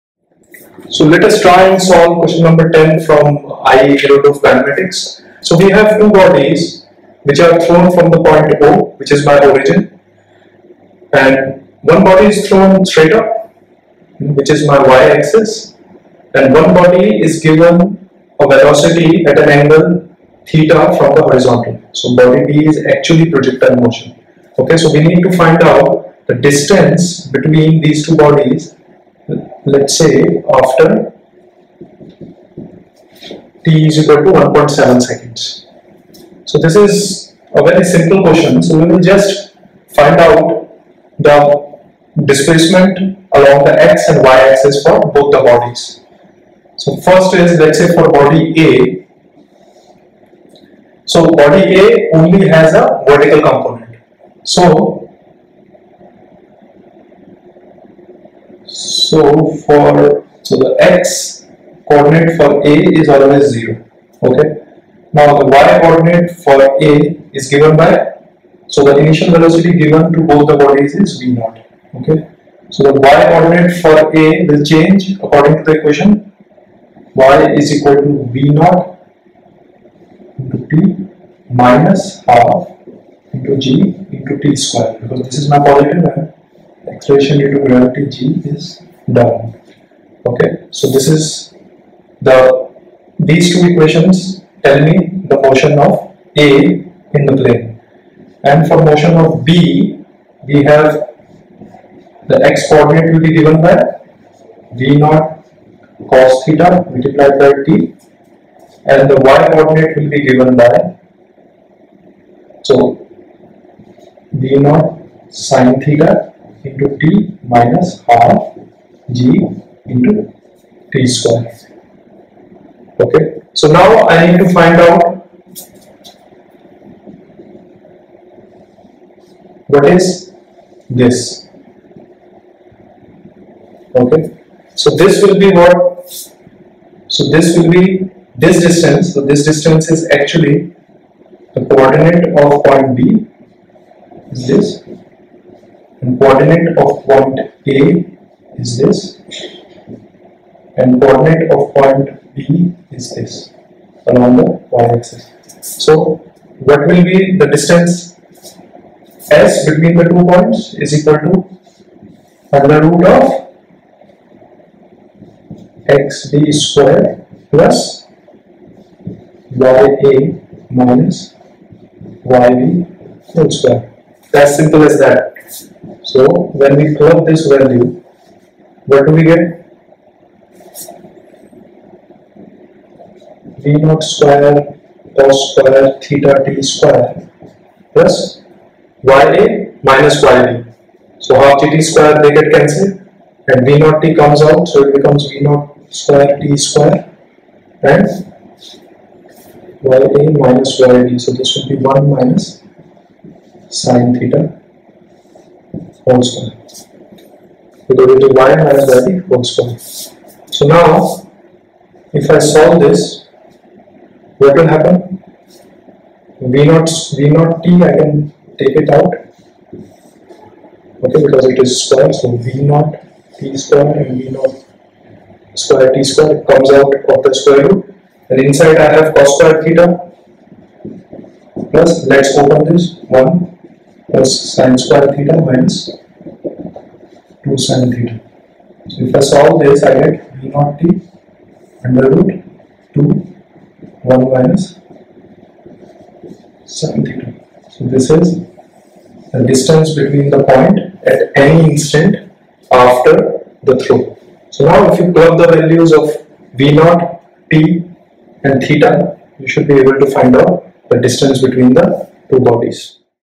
Q10 - Irodov - Kinematics
IIT Masterclass solves some good conceptual Problems from the Physics all time classic for IITJEE Preparation.
Please try solving yourself first and only see this after you have checked out all HC Verma Solutions on the playlist.